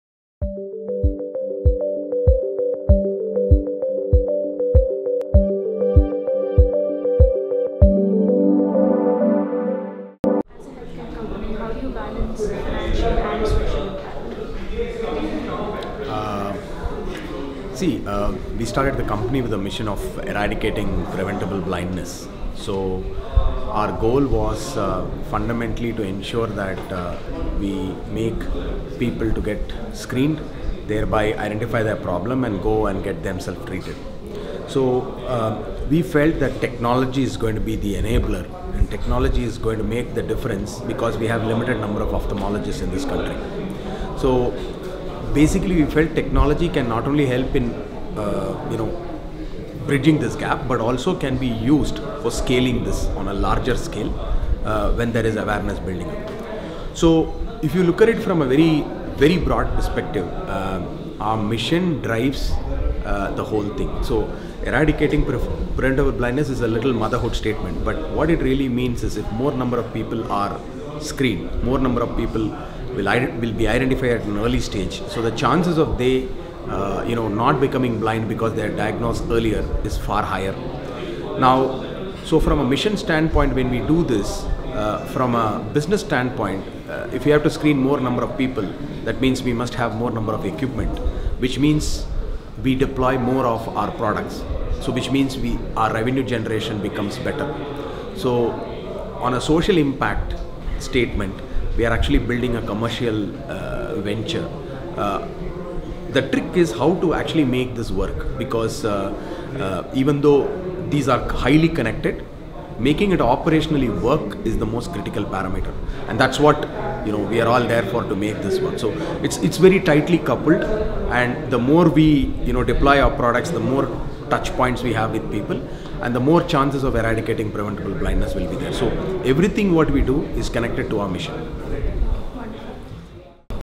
K Chandrasekhar, Founder & CEO, Forus Health
How can you balance social impact and financial growth?